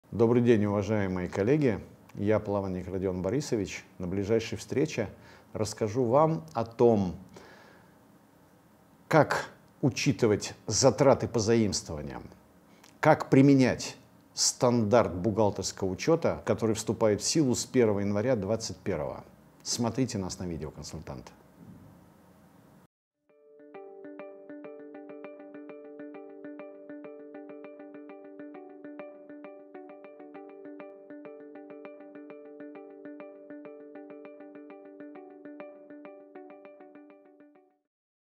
Смотрите на Видео.Консультант семинар «Как учреждениям учитывать затраты по заимствованиям»
Стандарт бухучета госфинансов «Затраты по заимствованиям» нужно применять при ведении учета с 1 января 2021 г. Лектор расскажет, для кого разработан новый стандарт. Разъяснит, как учреждениям отражать затраты по заимствованиям.
Лектор – Плавник Радион Борисович, главный эксперт по бюджетному учету Международного института сертифицированных бухгалтеров и финансовых менеджеров, аттестованный преподаватель ИПБ России, член Палаты налоговых консультантов России.